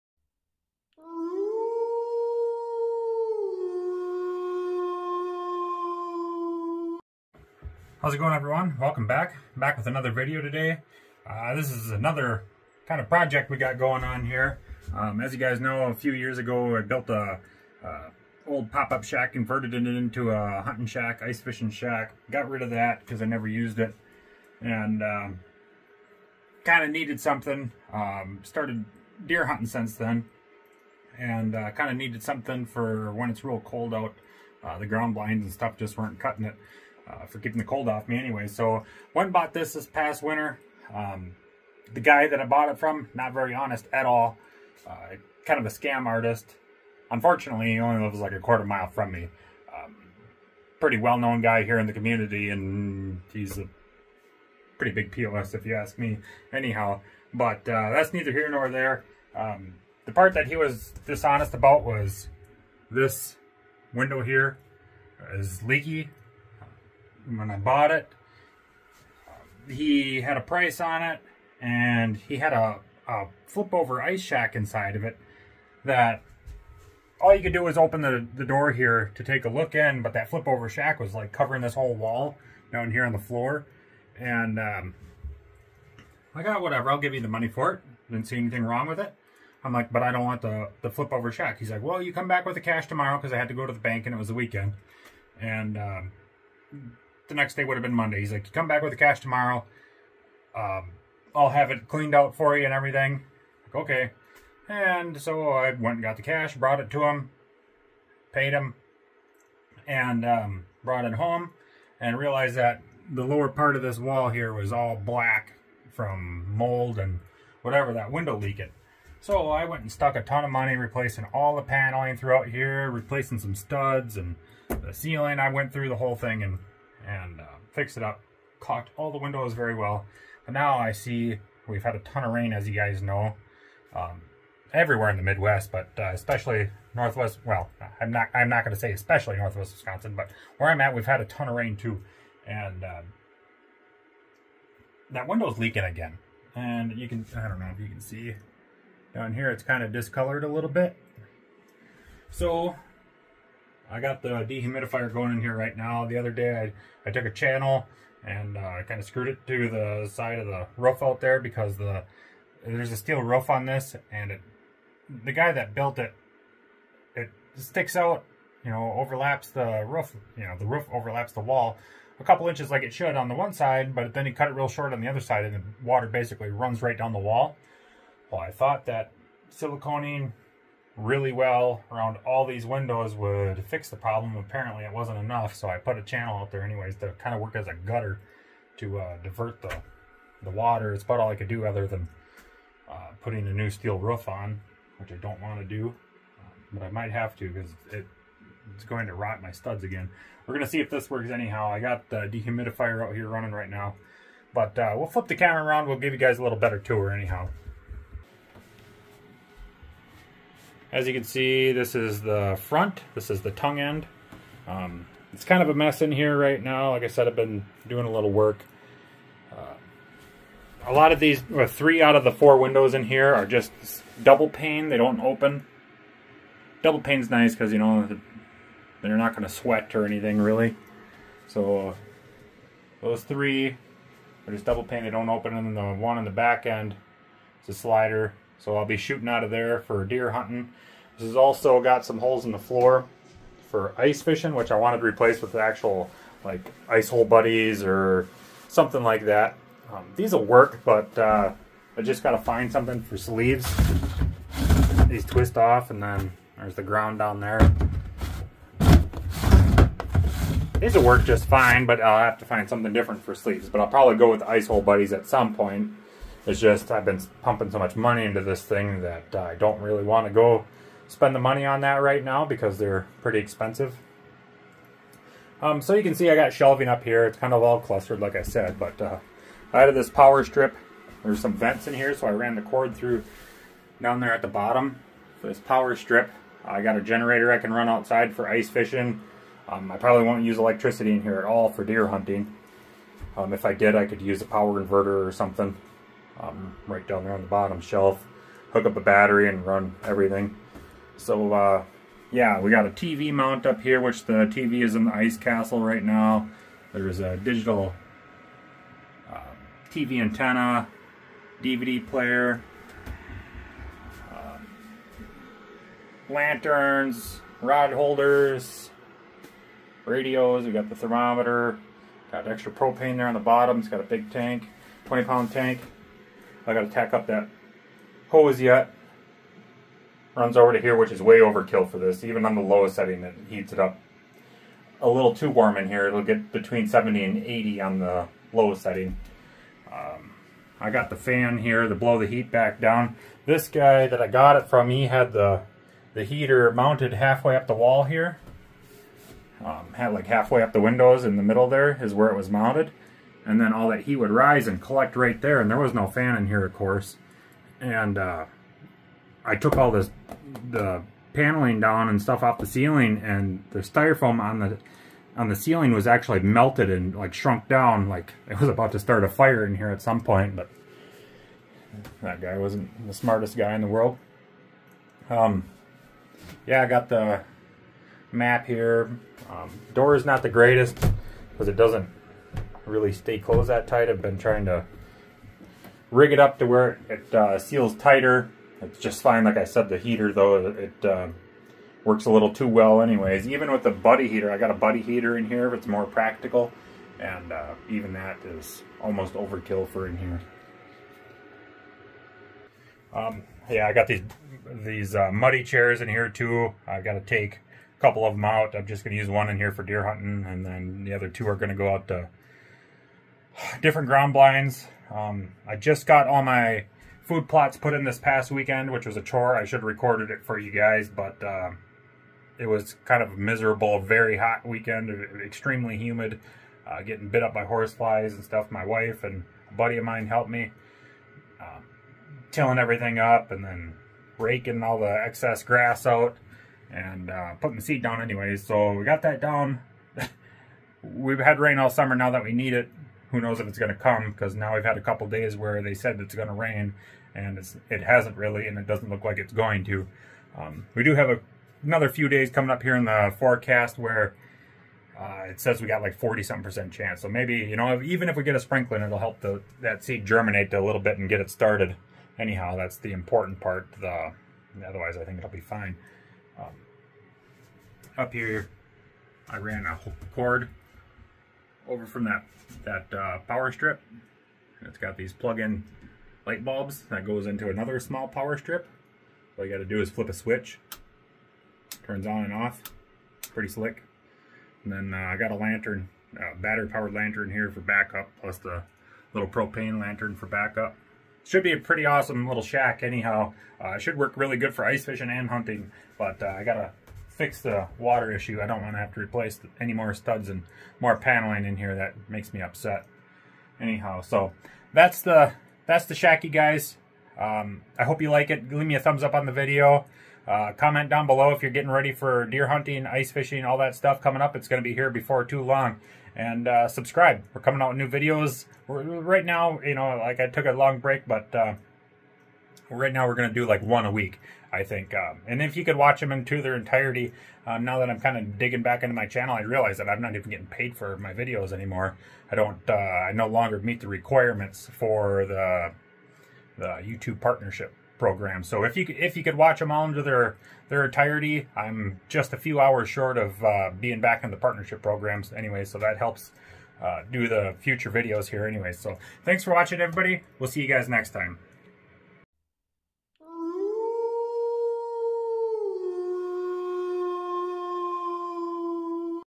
Another Hunting and Ice Fishing Shack Build
I bought a shack from a dishonest person, now I have to refurbish it.